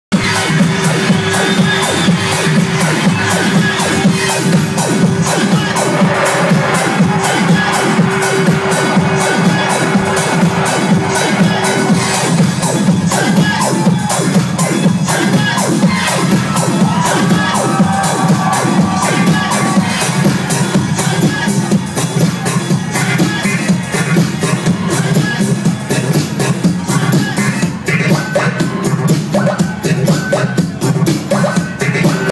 Milan serving it on stage before Escort's live show
Dwayne Milan killing it on stage at the Spank Production
live event @ Williamsburg Hall Of Music.
Live performances by Escort and Midnight Magic.
a magical night.